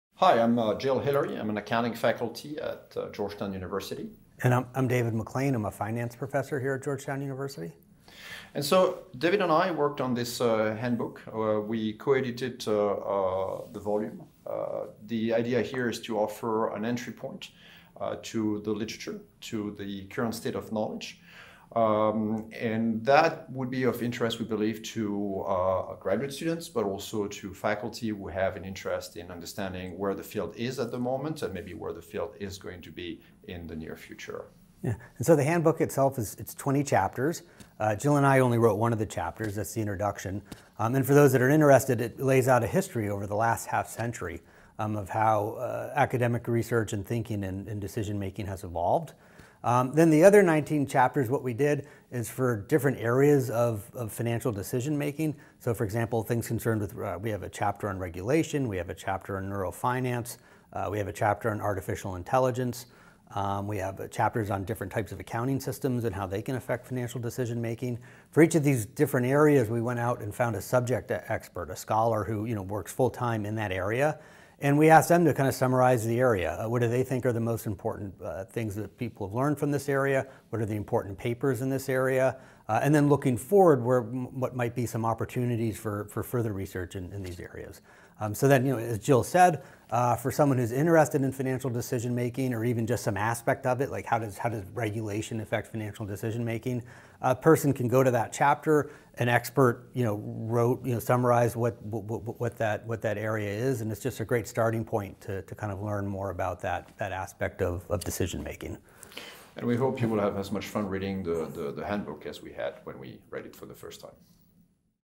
Edward Elgar Publishing - Handbook of Financial Decision Making
Gilles Hilary and David McLean discuss their new book Handbook of Financial Decision Making